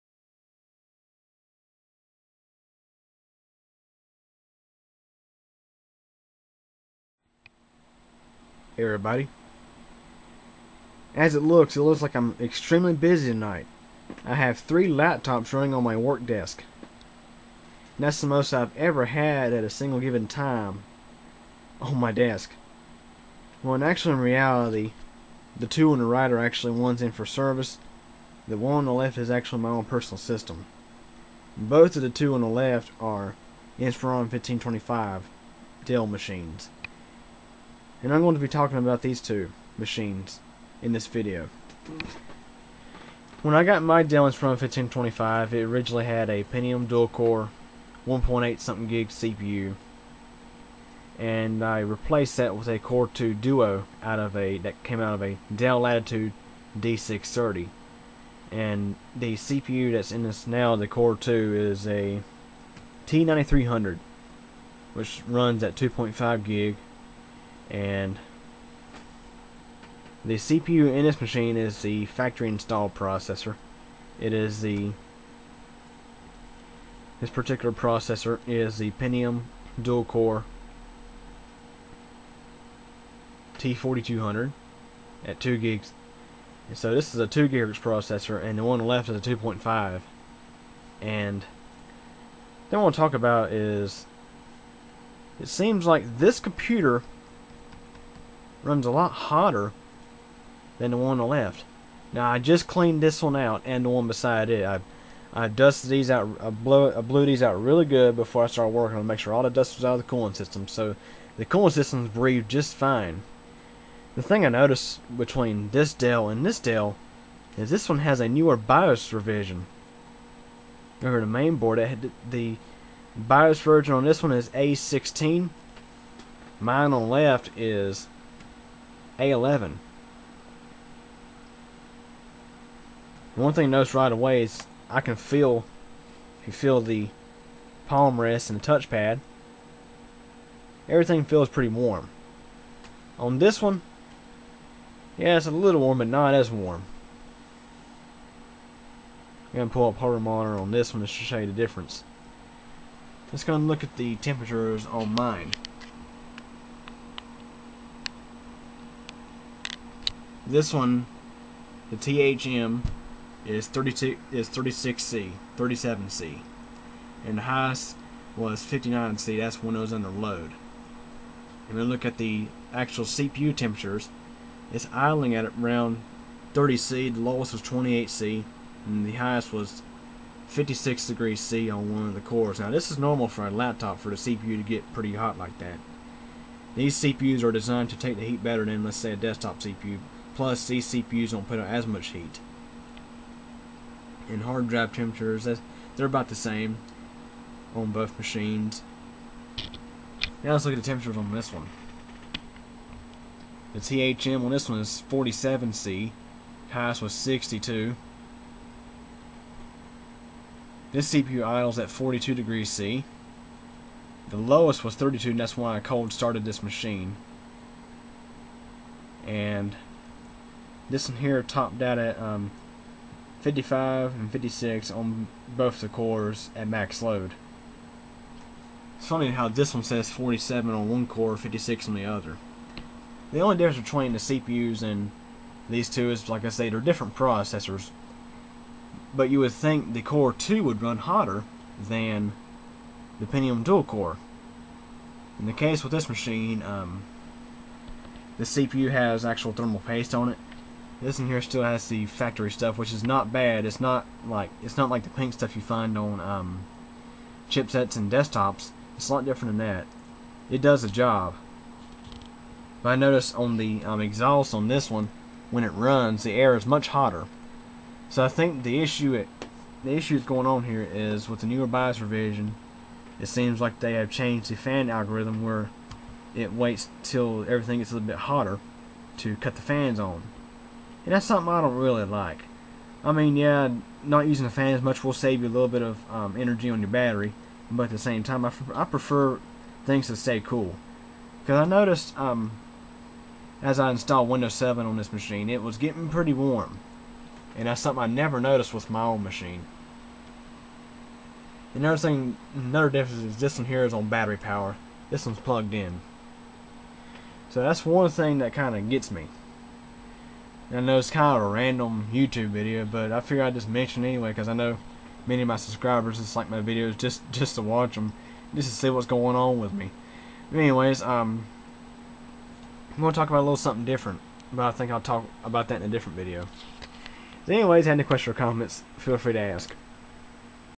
Difference in Operating Temperatures on 2 Dell Inspiron 1525 PCs
It seems like the newer BIOS revision A16 on a Dell Inspiron 1525 in for service makes the laptop run hotter than my own 1525 with BIOS A11 installed, even though my 1525 has a higher-end CPU running at a higher clock speed, compared to the 1525 in for service.